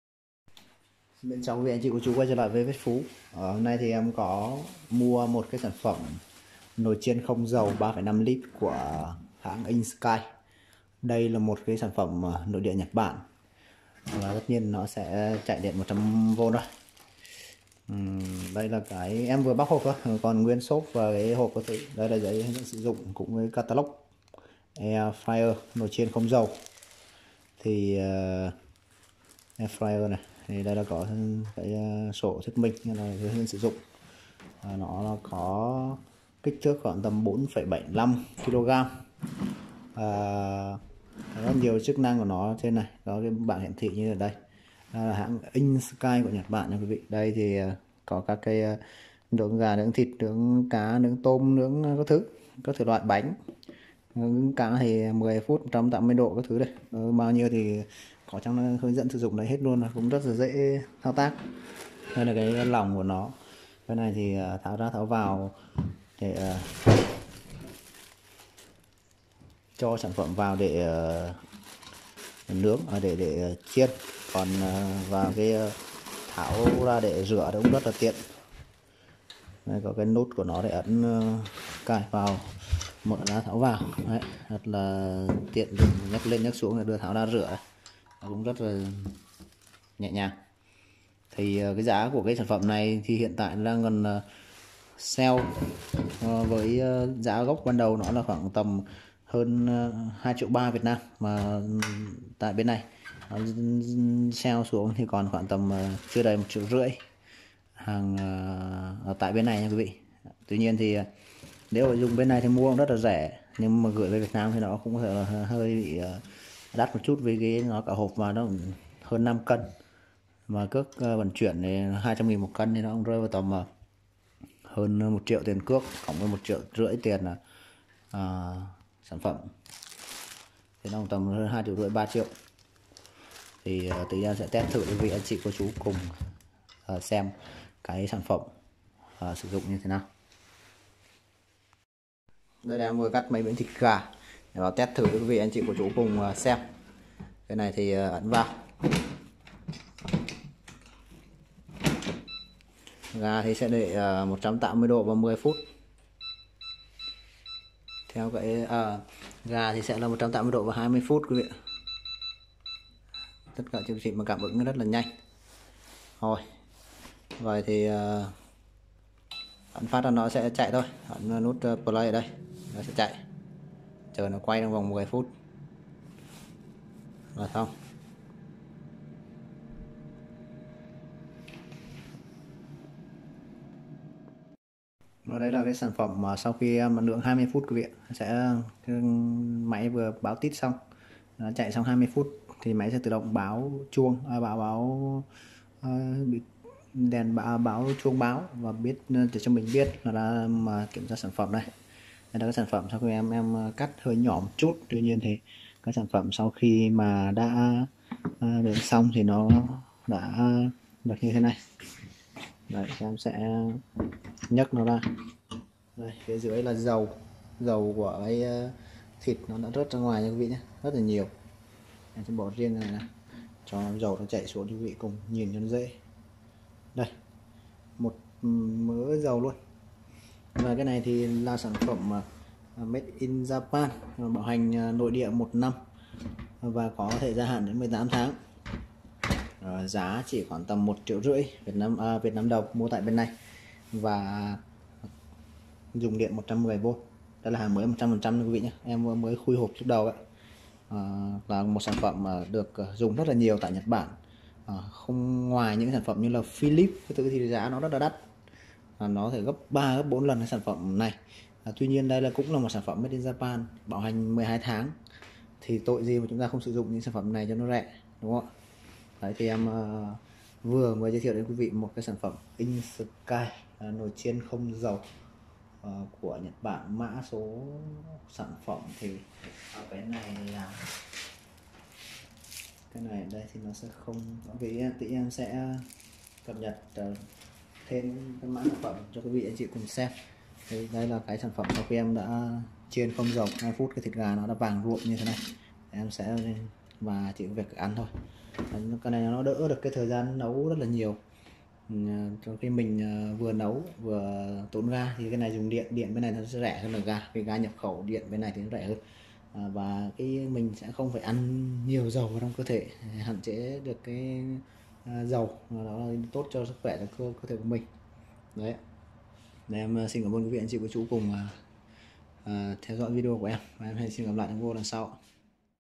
Đập hộp nồi chiên không dầu nội địa Nhật giá chỉ hơn 1 triệu VND/ Viết Phú JP
Đập hộp nồi chiên không dầu innsky nội địa Nhật giá chỉ hơn 1 triệu VND/ Viết Phú JP